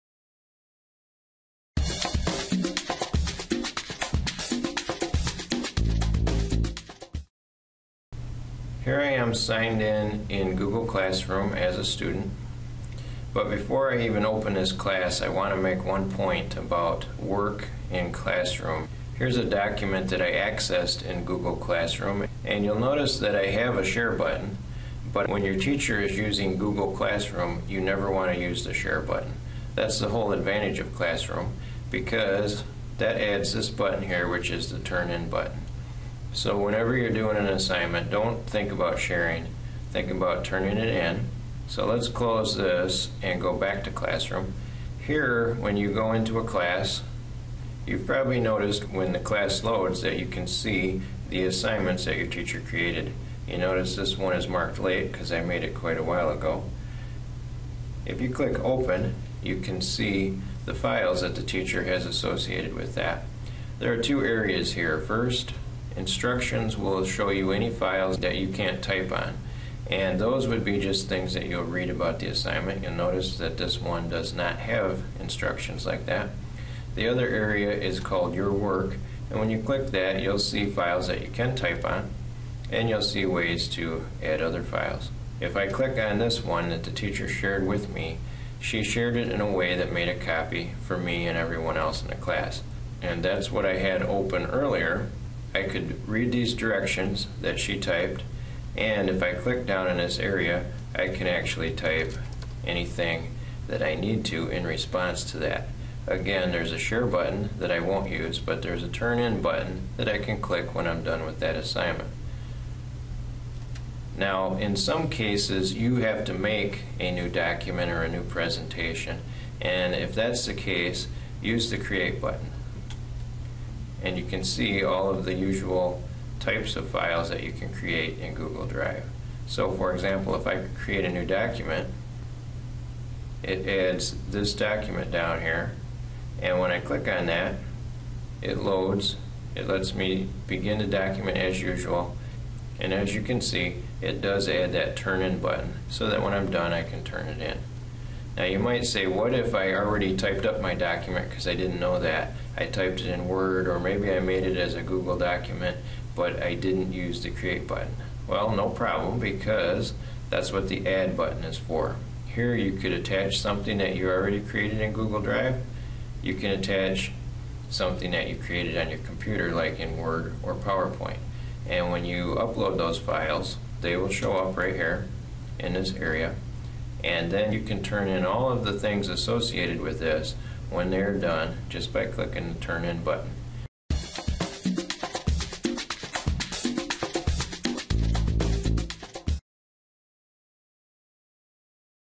Turning in Assignments in Google Classroom
Here I emphasize the use of Turn In instead of Share. I also point out the three ways of making an assignment:
-Type on the document the teacher attached.
-Create a new one.
-Upload a file.